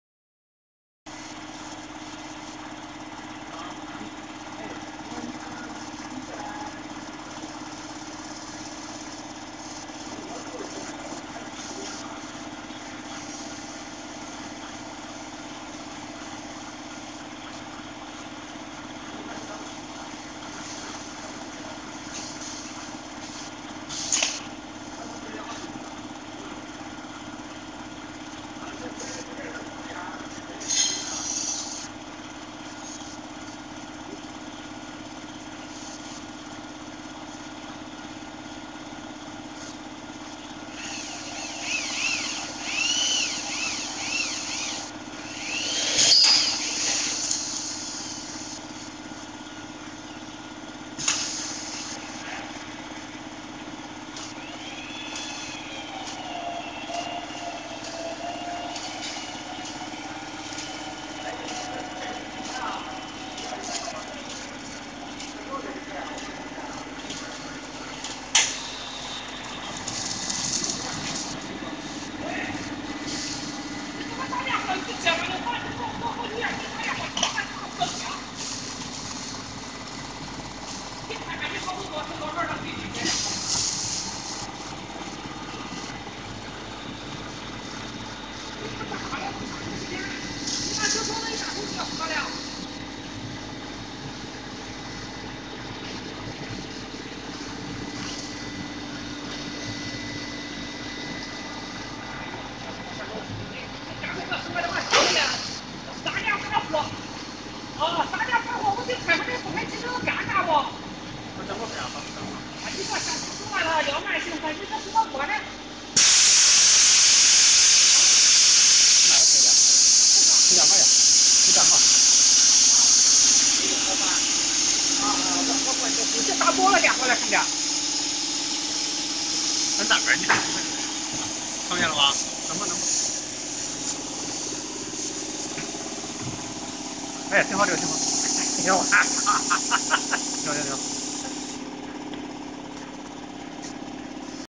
Low level online pallet wrapper with the top film dispenser
This online pallet stretch wrapper machine has the low level roller conveyor on the turntable which is customized in order to connect with the customer’s existing line that has the same low level. The machine adopts the top film dispenser which can apply a layer of thick PE film onto the top of the pallet loads. The whole equipment is able to make fully sealed wrapping for the pallet and skid loaded cargo. It has the ability to prevent the damage or the contamination of rain and dust. The wrapped goods even can be placed outside for a long time if the warehouse is full. Pallet stretch wrapping machine/ pallet wrapper/ Pallet packer/ Film dispenser.